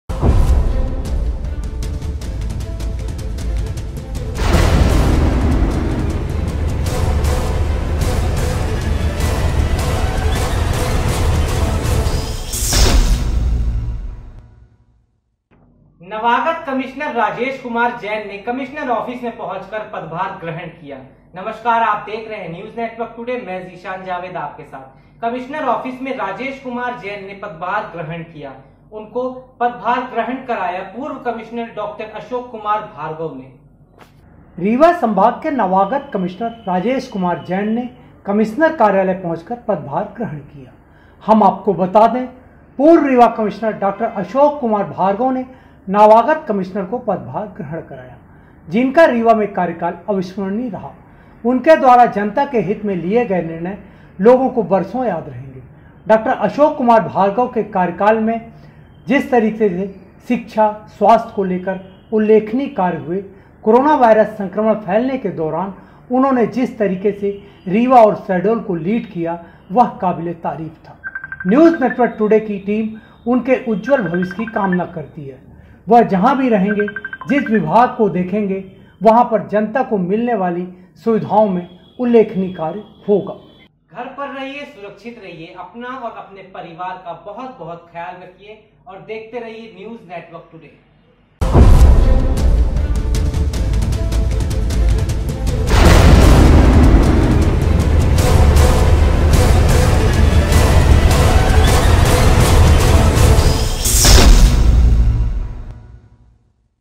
कमिश्नर राजेश कुमार जैन ने पदभार ग्रहण किया पूर्व कमिश्नर डॉ अशोक कुमार भार्गव की विदाई ||NNT
कमिश्नर अशोक कुमार जैन ने पदभार ग्रहण किया पूर्व कमिश्नर डॉ अशोक कुमार भार्गव की विदाई ||NNT||